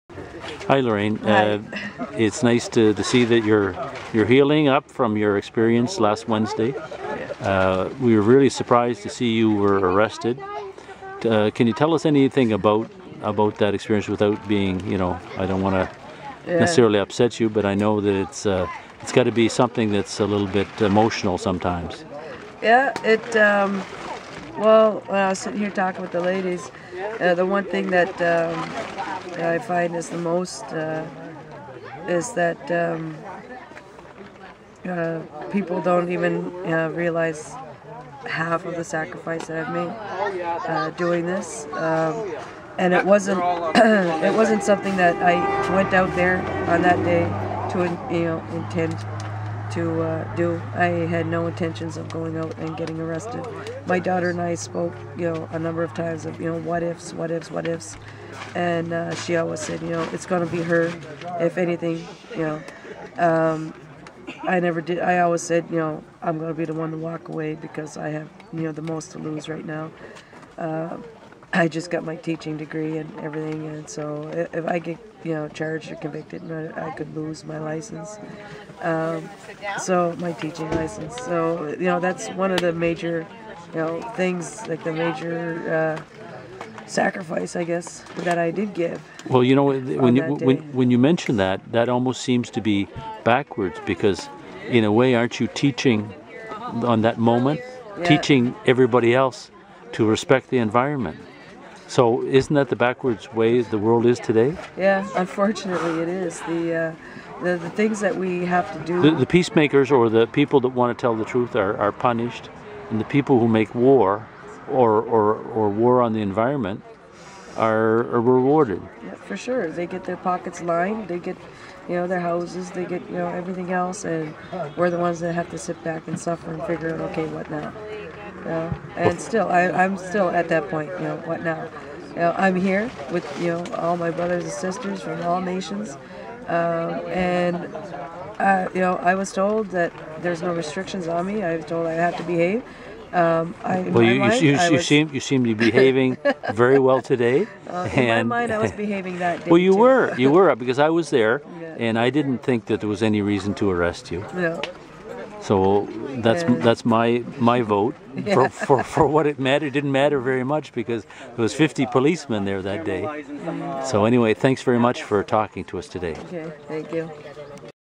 A conversation with Lorraine Clair
Lorraine Clair, from the native community was arrested on June 5, 2013 and received nerve damage.  I ask her to reflect on that experience today June 13th at the encampment at the corner of Rte 126 and Rte 116 west.